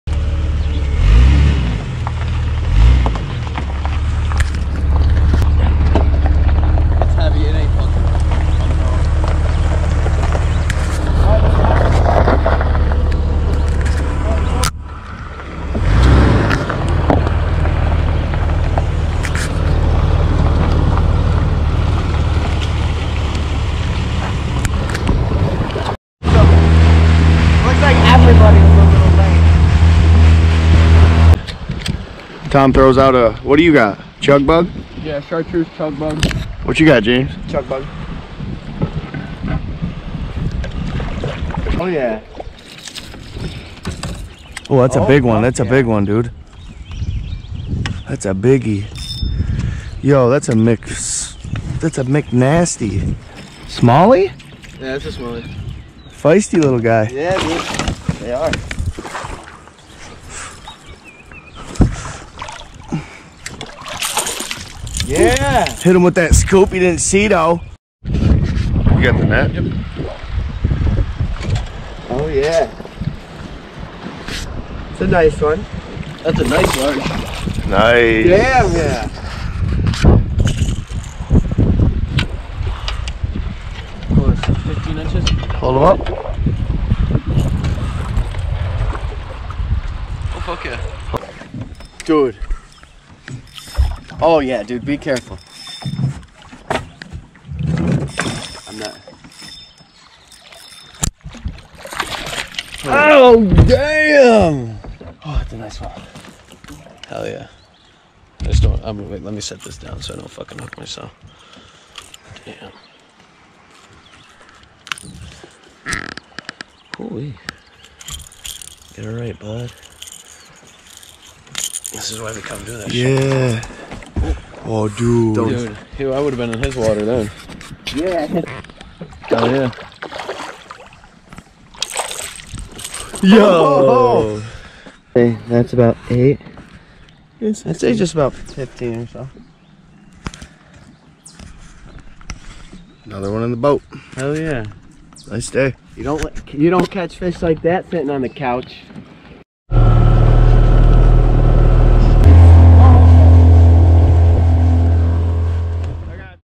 FISHING for SMALLMOUTH BASS in HEMLOCK LAKE.
Just a few early bass . Catch and release , still fun !